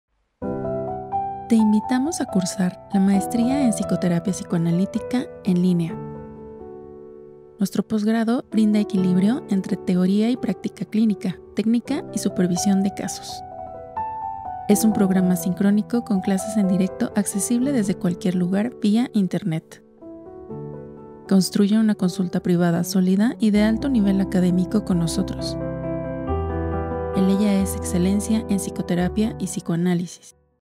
Maestría en Psicoterapia Psicoanalítica. Inscríbete_
Centro Eleia, sólo Psicología, ofrece su Maestría en Psicoterapia Psicoanalítica en línea. Estudia y dialoga con expertos, dos veces por semana, desde cualquier lugar. Estudia todas las perspectivas psicoanalíticas.
Inscripciones Abiertas